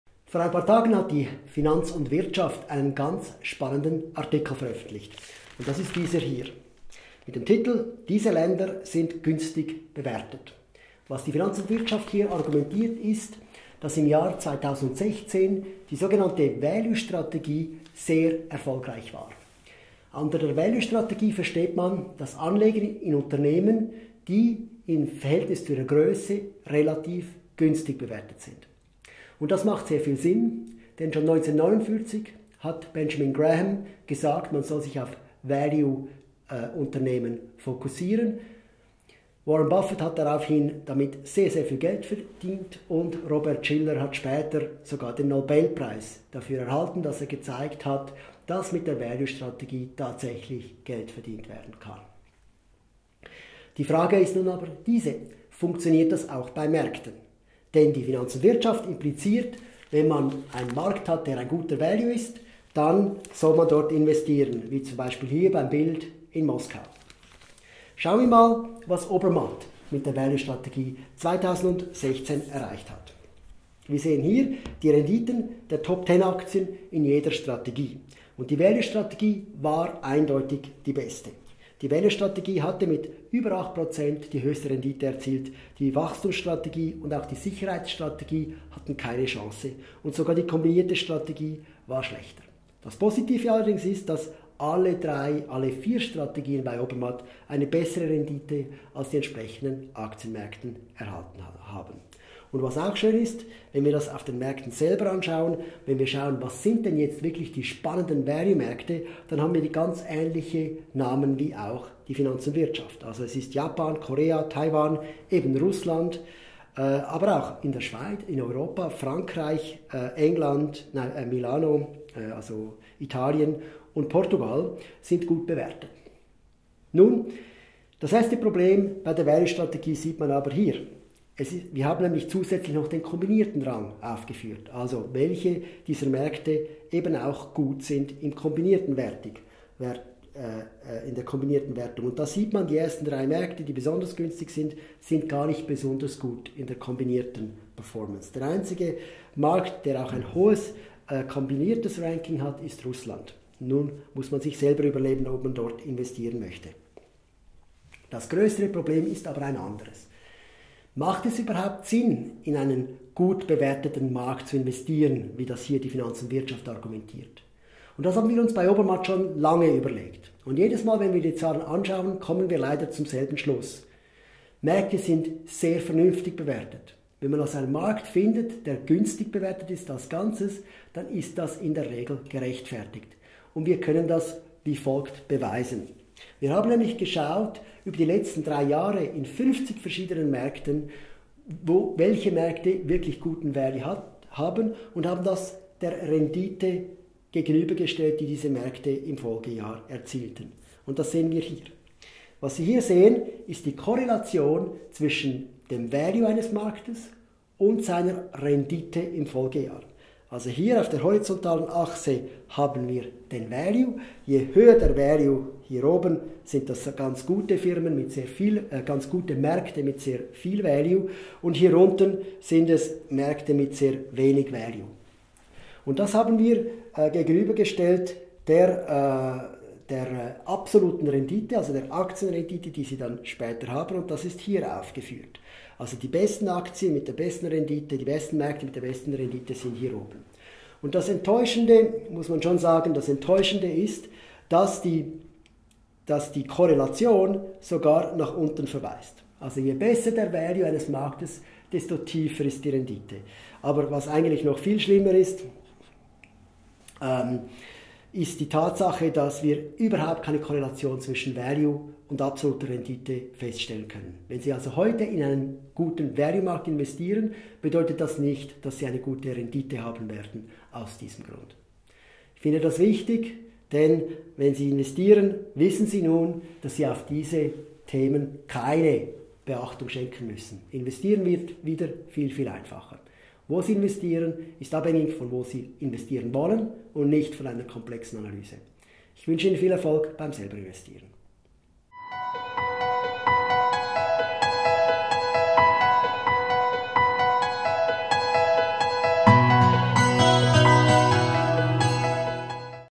Irrtum in F&W: “Value” Märkte helfen Aktionären nichts
Kürzlich hat die Finanz & Wirtschaft empfohlen, in “Value” Märkte zu investieren, denn die Value Strategie war 2016 die beste Anlagestrategie. Ein Bild vom verschneiten Moskau unterstreicht im F&W Artikel das Studien-Resultat, denn Russland sei aktuell ein Value Markt und man solle dort investieren.

Bei der Value Strategie investiert man in Firmen, die im Vergleich zu ihrer Grösse günstig bewertet sind. Der US Investment-Vordenker Benjamin Graham hat das 1949 erstmals postuliert, Warren Buffett hat damit eine Menge Geld gemacht und Robert J. Shiller hat für den Nachweis, dass die Value Strategie auch tatsächlich funktioniert, kürzlich den Nobelpreis erhalten.

Auch bei uns ist die Value Strategie der zentrale Anker. Die Obermatt Top 10 Value Aktien waren letztes Jahr denn auch bei uns - wie bei der F&W - die beste Wahl. Die Value Top 10 Aktien schlugen deren 56 Vergleichsindexe im Durchschnitt um stolze 8.6%. Value Aktien waren in 35 von 56 Märkten besser als die Indexe, stolze 63%. In diesem Aspekt sind wir mit der F&W einig. Wir sind auch darin einig, dass Russland ein Value-Markt ist, allerdings haben bei uns Japan, Südkorea und Taiwan einen noch leicht besseren Value

Das Problem beim F&W Artikel ist denn auch nicht die Analyse, sondern die Schlussfolgerung. Muss man jetzt in Value-Märkte investieren?

Das ist nicht der Fall, denn schon seit Jahren sehen wir immer wieder, dass die Value-Strategie für die einzelne Aktienauswahl zwar zuverlässig funktioniert, aber bei ganzen Märkten gibt es zwischen Markt-Value und anschliessender Rendite leider keine Korrelation.

Wie man auf der Abbildung sieht und wir im Video zeigen, hat die Rendite im Folgejahr nichts mit dem “Value” des Markts im Jahr davor zu tun, in unserem Datensatz ist die Korrelation sogar leicht negativ. Die Investition in ein Value-Markt nützt also nichts.

Wie lässt sich das erklären? Unser Erklärungsvorschlag ist, dass auf der Ebene Markt Fehler bei Bewertungen einzelner Unternehmen ausgeglichen werden. Ein hoher Value im russischen Markt heisst dann eben nur noch, dass dieser eher weniger Wert ist als die anderen und eben nicht, dass sich dort gute Anlagechancen ergeben. Das kann zwar vorkommen, ist aber nach unseren Analysen ein reiner Zufall.

Das sind gute News für Anleger, denn sie müssen sich um die komplizierten Marktanalysen gar nicht kümmern. Die Anlageregel bleibt einfach: Investiere regelmässig kleine Häppchen und schere dich nicht um die Märkte und Branchen, die gerade “heiss” seien.
Verweis: F&W: “Diese Länder sind günstig bewertet”, Seite 17, 18. Januar 2017